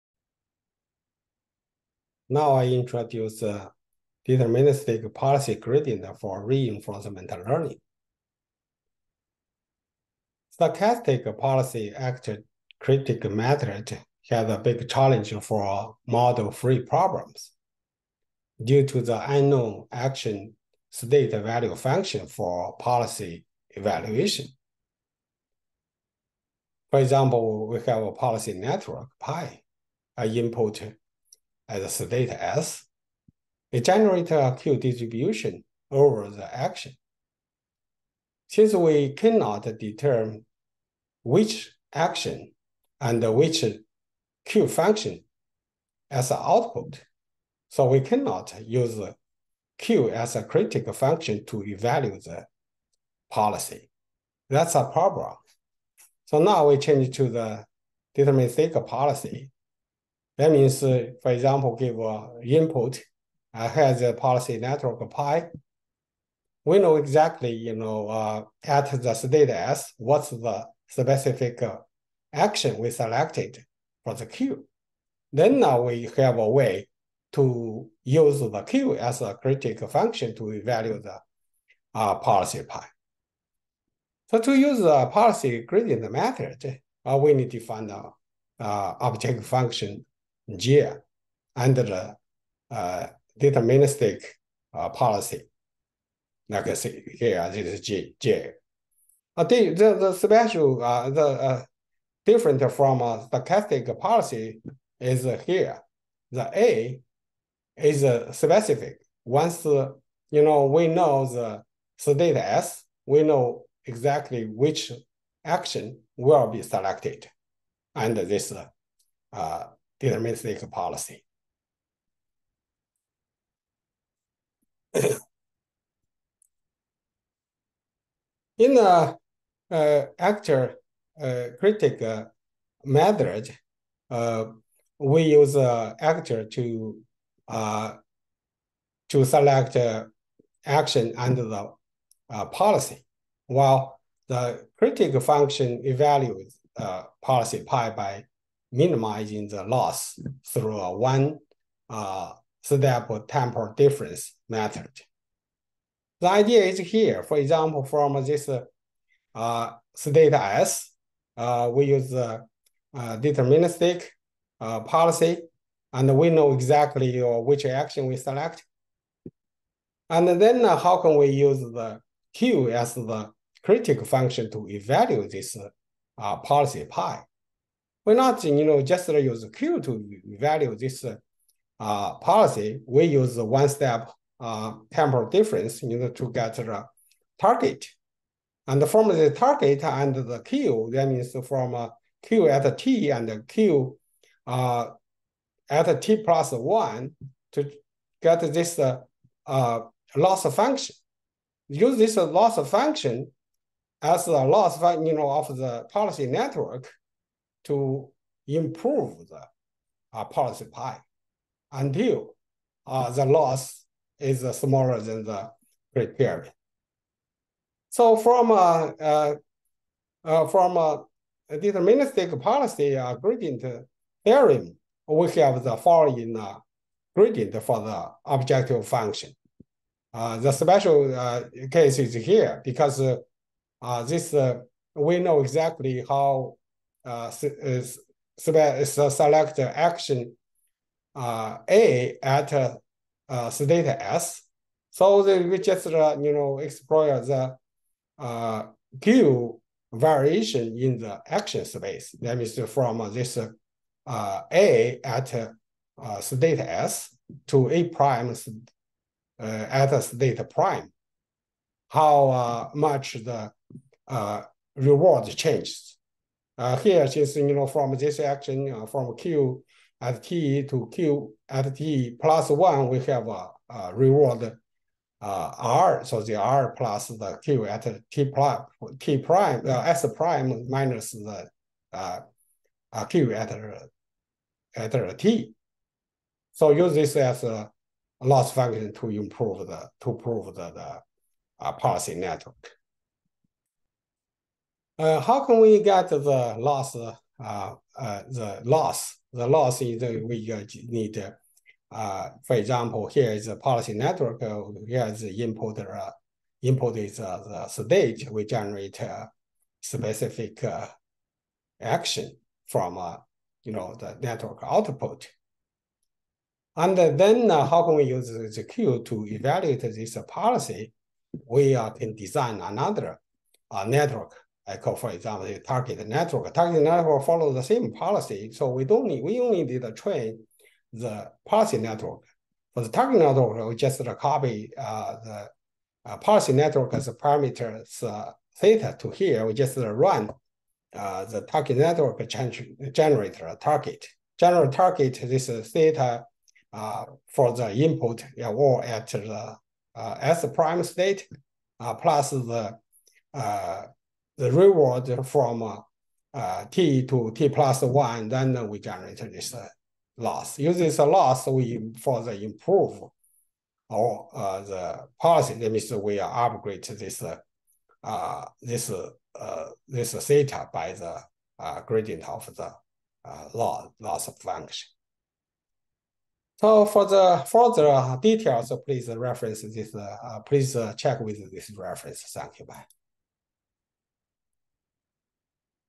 Deterministic policy gradient theorem for reinforcement learning
Reinforcement learning, deterministic policy, stochastic policy, actor-critic algorithm